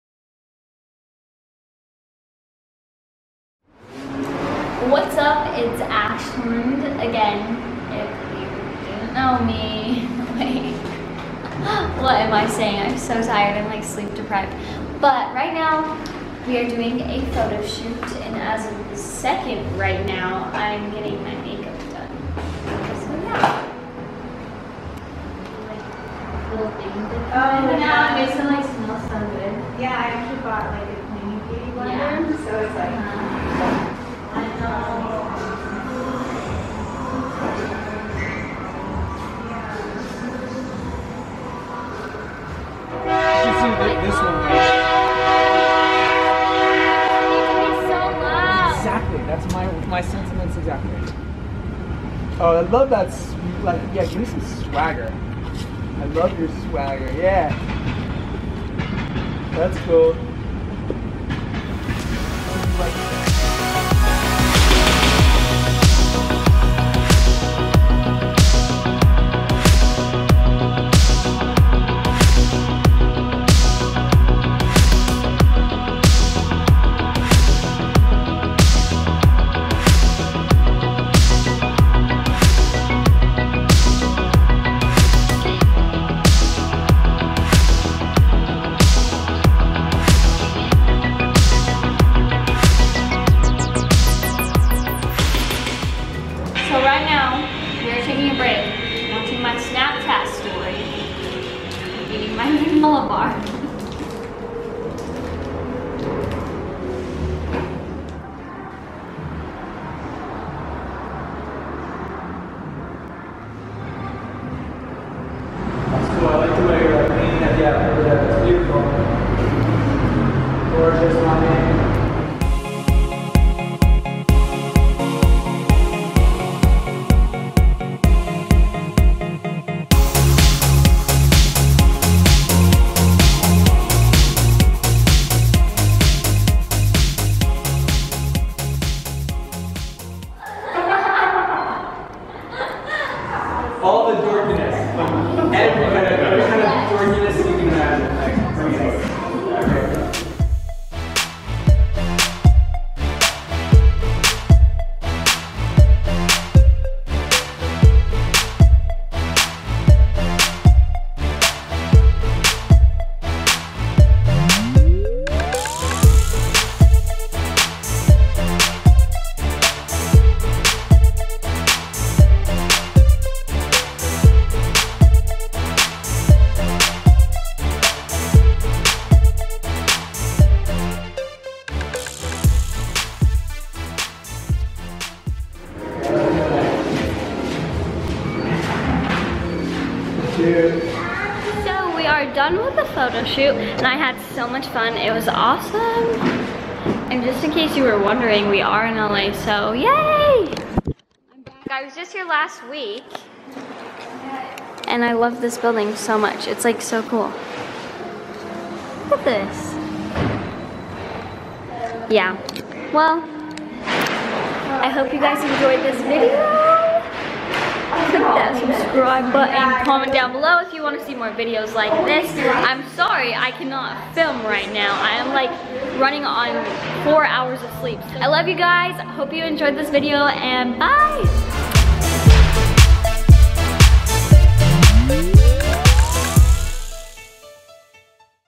Photoshoot - Wes Klain | Ashlund Jade
Hey guys! I had so much fun in LA doing a photoshoot with the amazing Wes Klain. This is a BTS of the photoshoot. Hope you enjoy the video. XOXO Ashlund

Connect with Ashlund Jade!
Musical.ly: ashlundjadeofficial